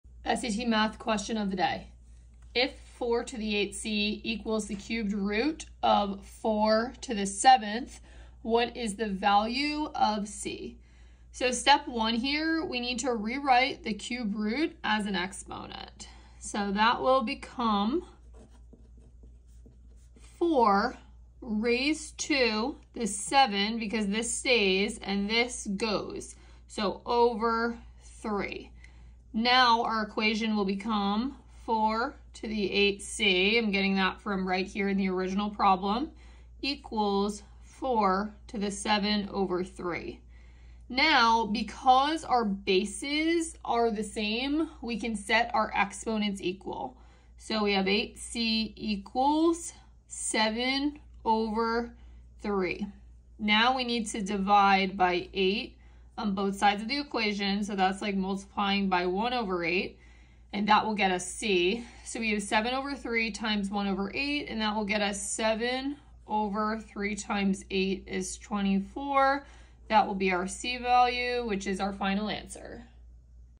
Can you solve this exponents question?
Get ready for your next SAT with our Math Question of the Day! In this video, we walk you through the problem step-by-step, explaining the reasoning and strategies that will help you solve it efficiently. Perfect for daily practice and boosting your confidence before test day.